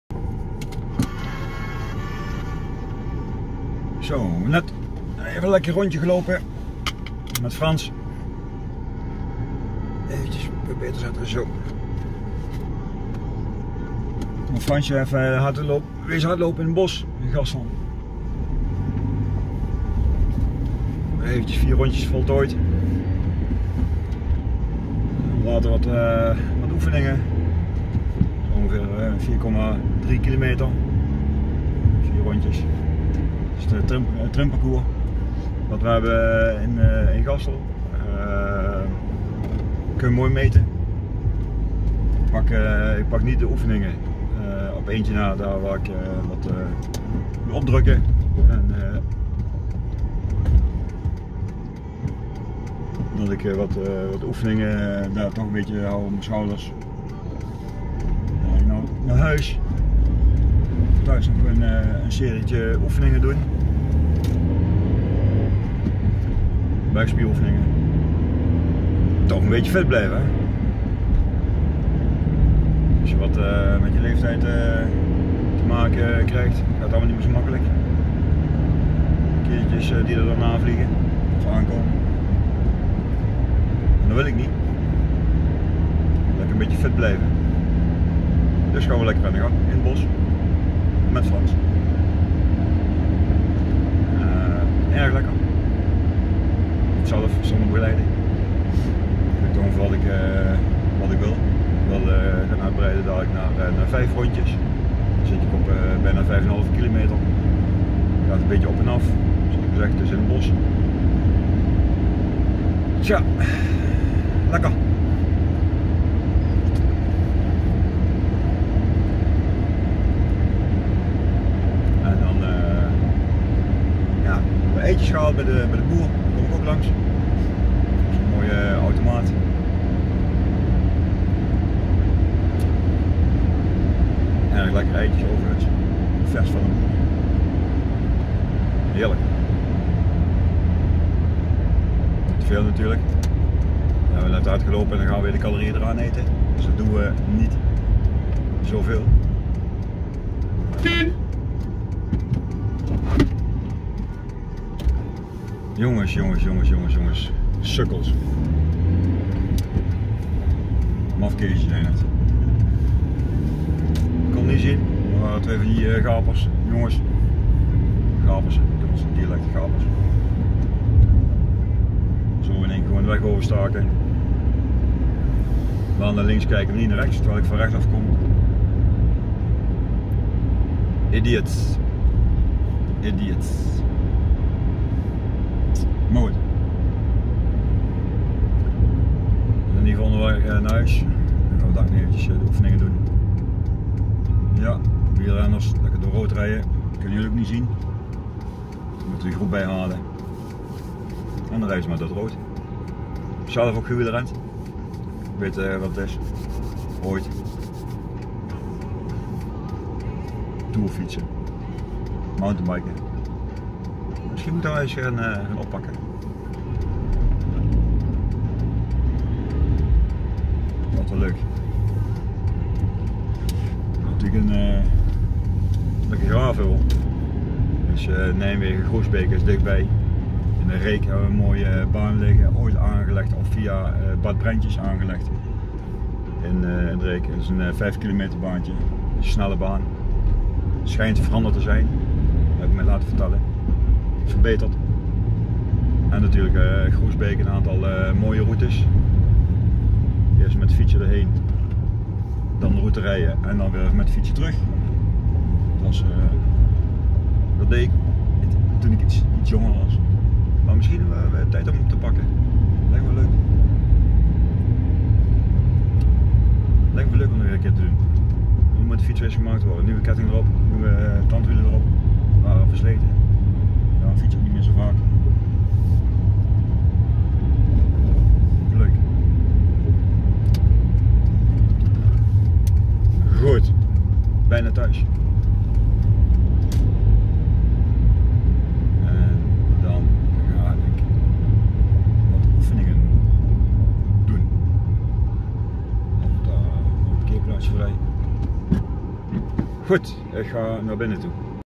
Hard lopen in Gassel met Frans #1
Hard gelopen met Frans in het Gasselse bos ongeveer 4,3km en Frans nog maar 3,2km. Dit omdat Frans nog maar net begonnen is met hardlopen.